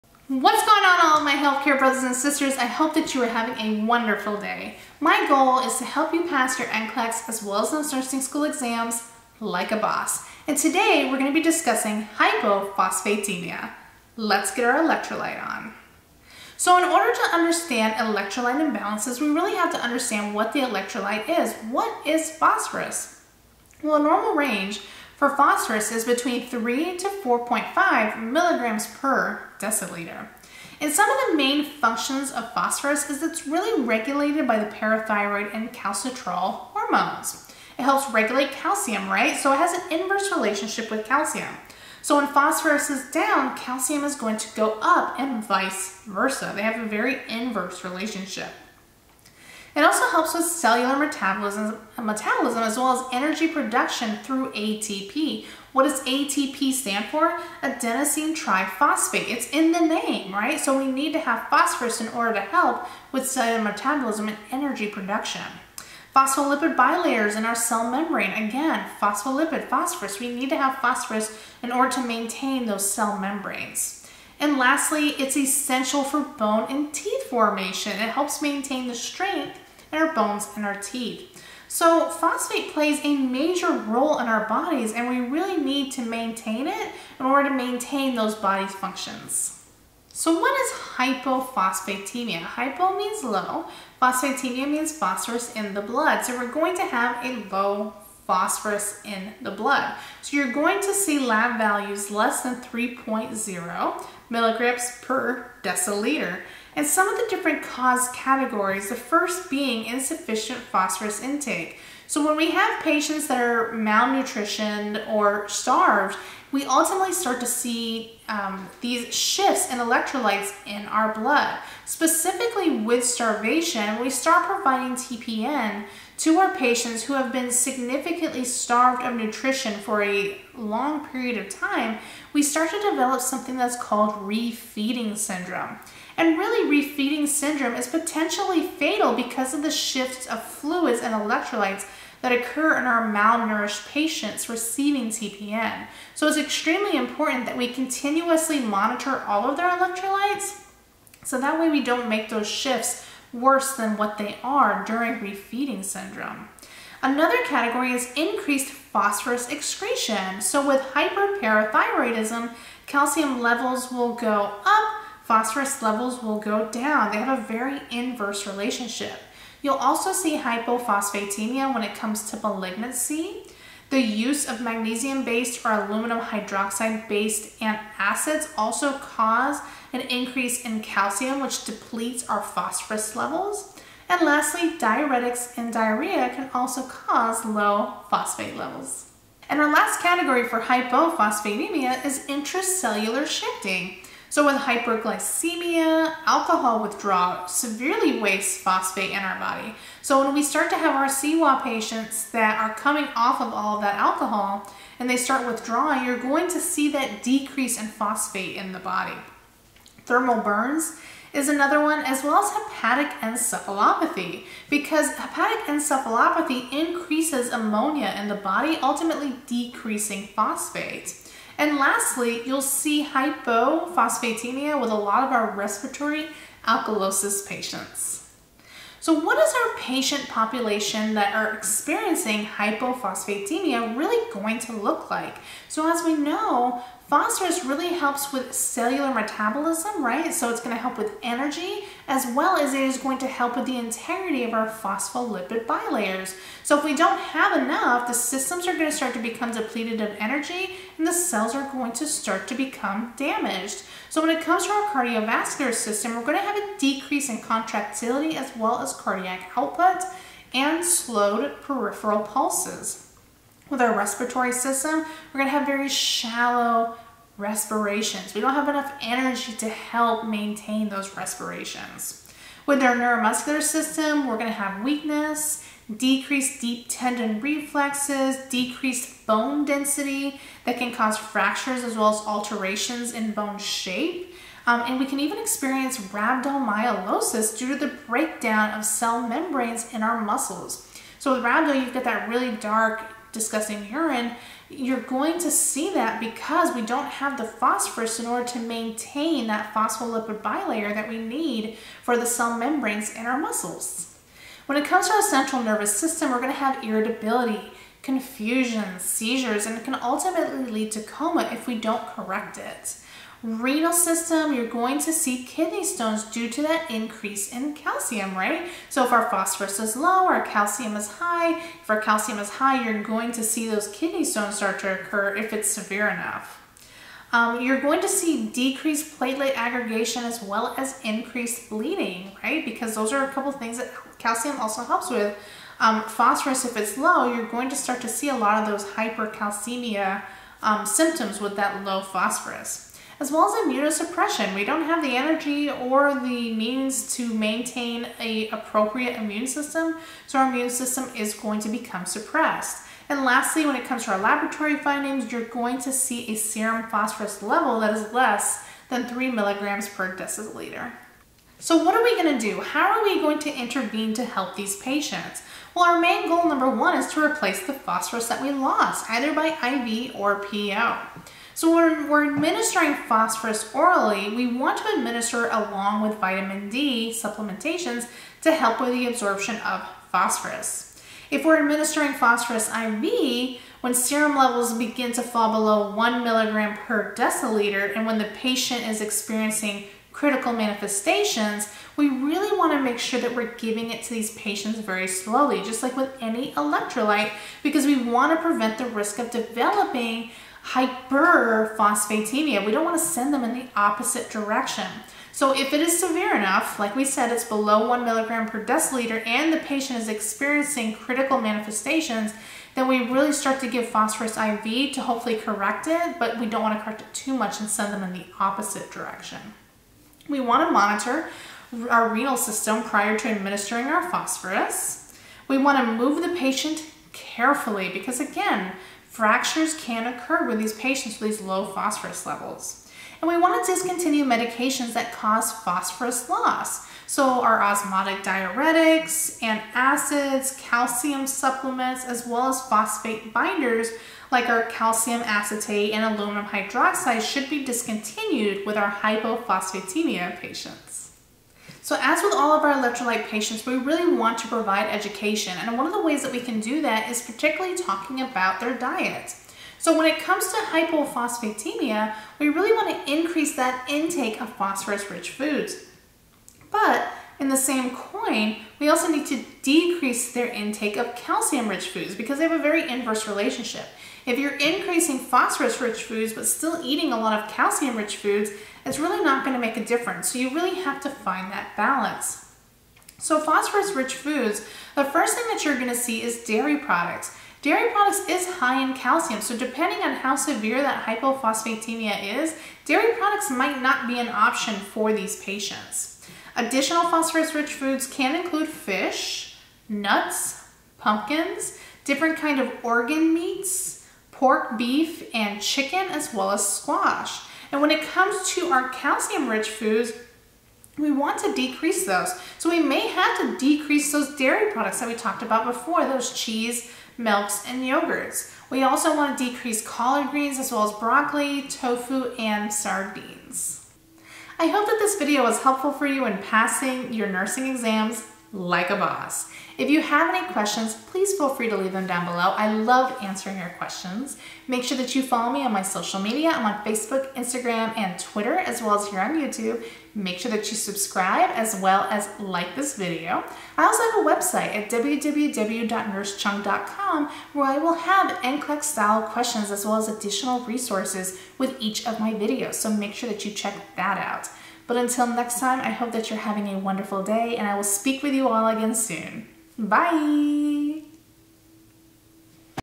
HYPOPHOSPHATEMIA (LOW SERUM PHOSPHORUS) | FLUID & ELECTROLYTE NCLEX NURSING EXAM LIKE A BOSS SERIES
Fluid and Electrolyte nursing review for NCLEX and nursing school exams. In this video, we discuss hypophosphatemia and its effect on the body systems.

This lecture explains the what phosphorus is, main functions of phosphorus, hypophosphatemia causes, hypophosphatemia patient presentation, hypophosphatemia interventions, and hypophosphatemia NCLEX TIPS.